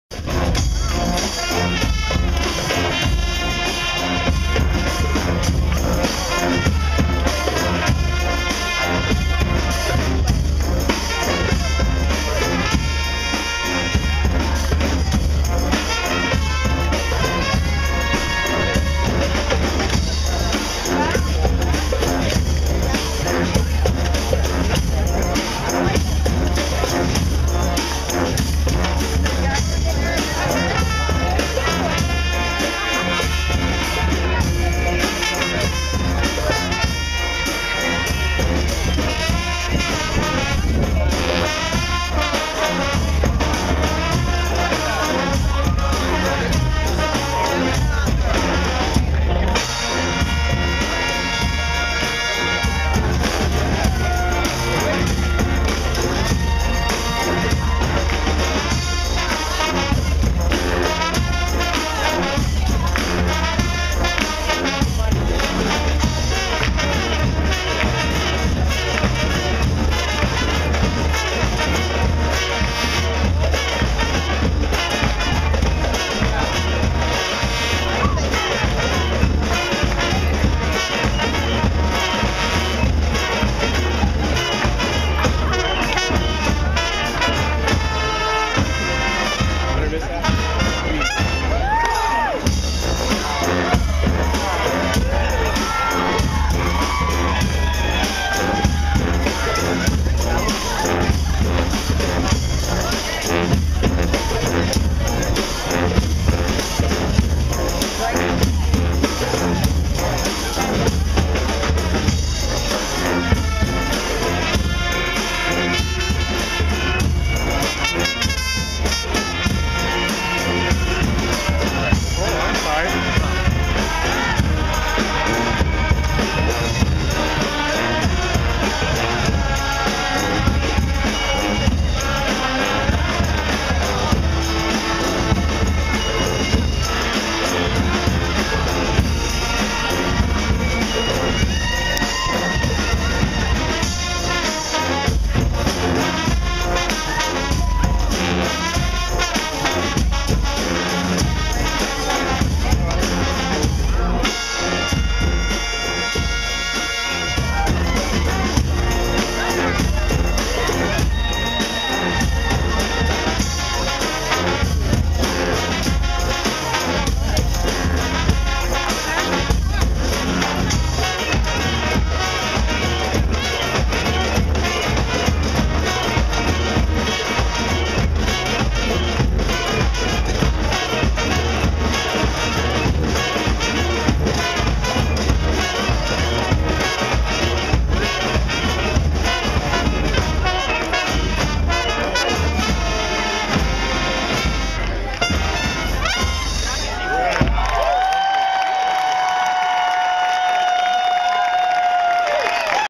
Honk! Minor Mishap
Austin's own Minor Mishap takes the stage and blows away the crowd.  Honk fest 2011.